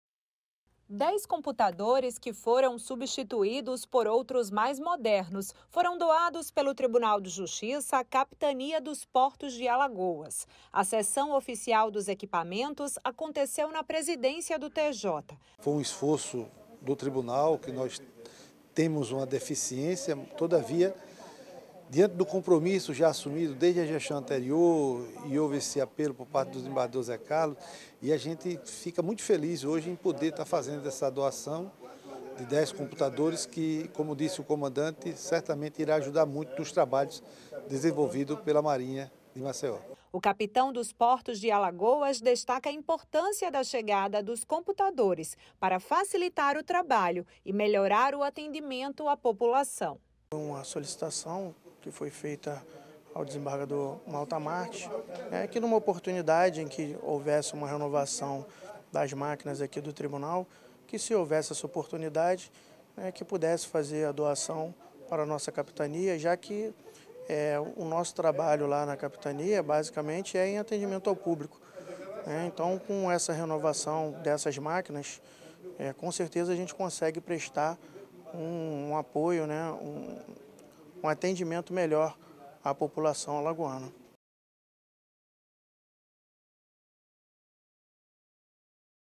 TJAL doa computadores à Capitania dos Portos de Alagoas
Equipamentos vão auxiliar no atendimento ao público realizado na Capitania